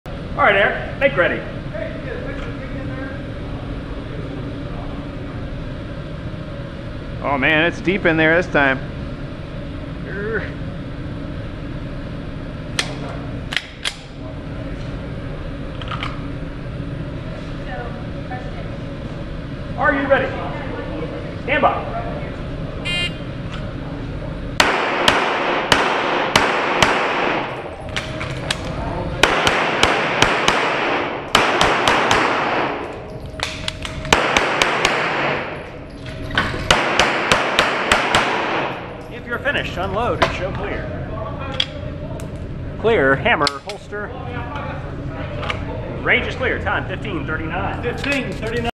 GPSL Stage 1 - Alley Strafe - Nov 08, 2016
Stage 1 at the November 08, 2016 Gwinnett Practical Shooting League match, which is held at Range, Guns & Safes in Norcross.

A fun stage with a lot of opportunity to shoot on the move. I probably could have improved my hits a little bit. The strong-hand through the port was questionable, too.

Glock 34
Dawson sights